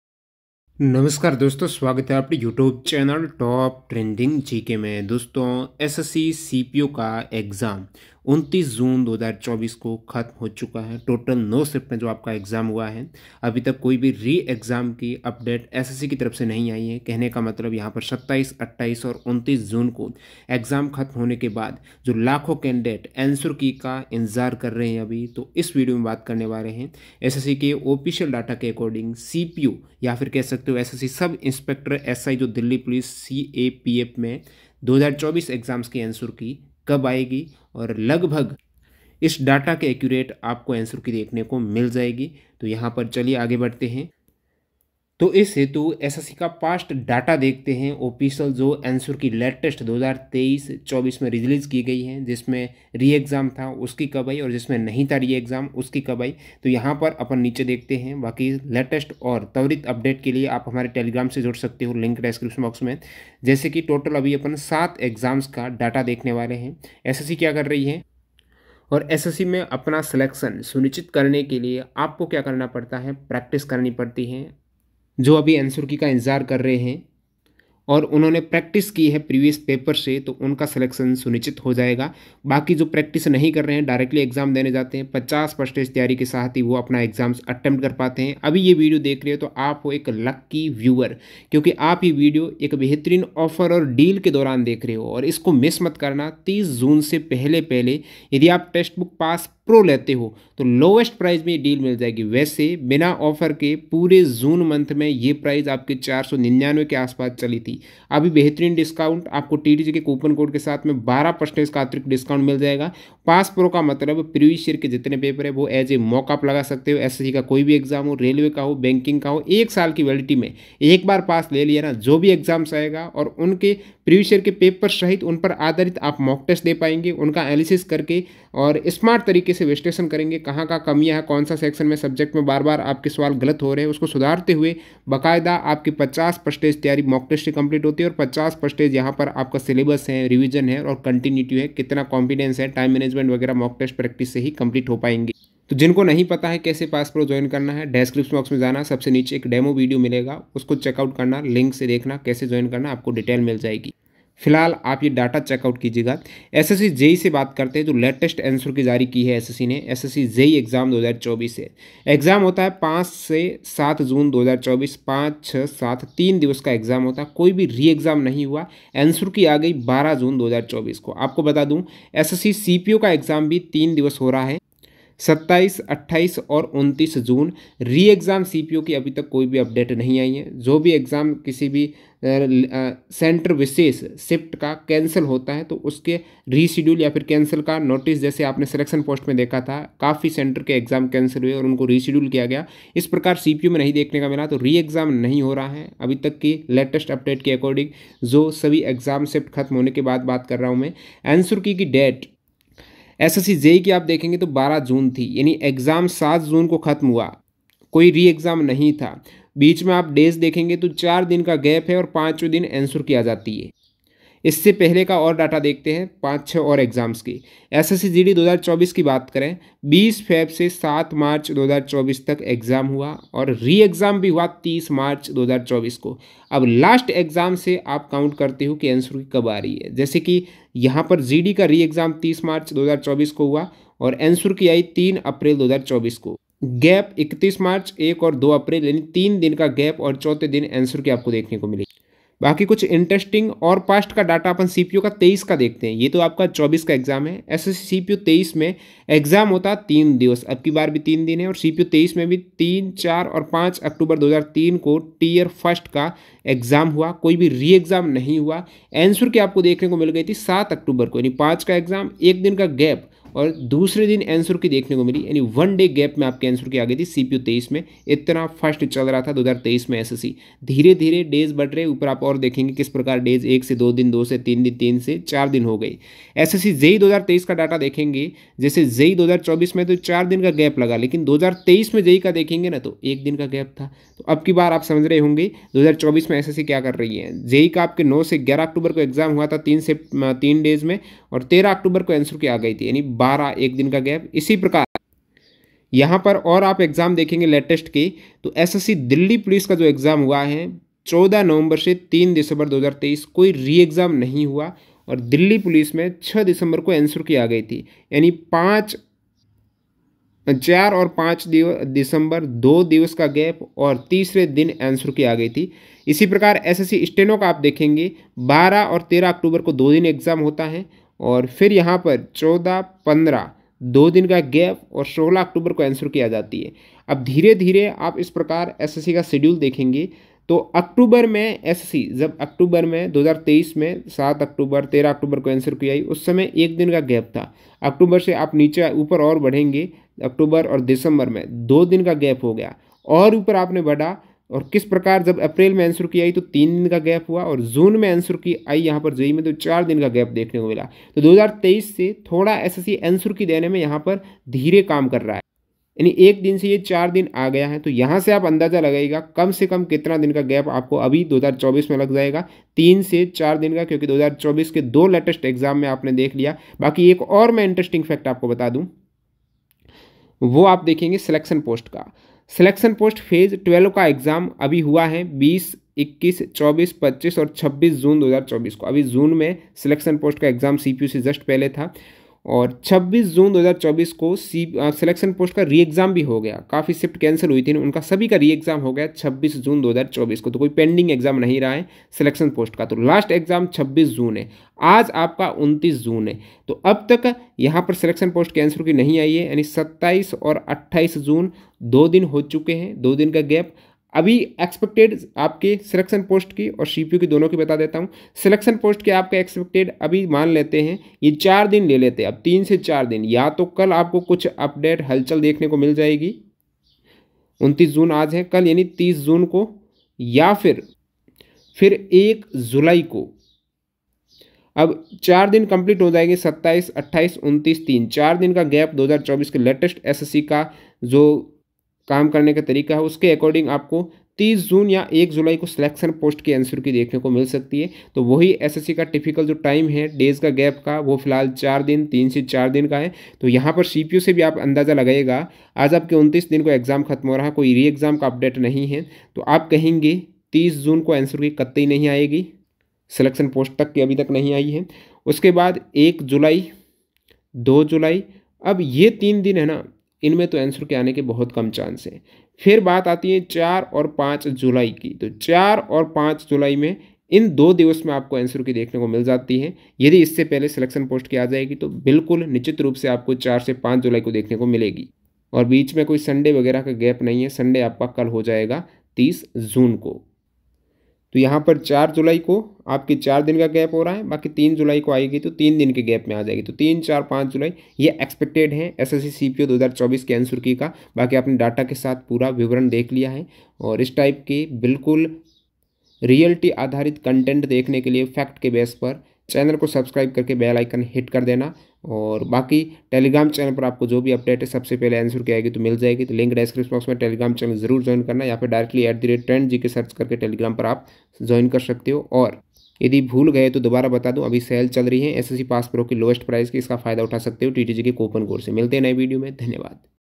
SSC CPO 2024 Answer Key Date | SSC CPO Exam 2024 Ans Key Kab Aayegi
SSC CPO Exam 27 ,28 and 29 June 2024 Answer Key Date. SSC CPO 2024 Ki Answer Key Kab Jari hogi.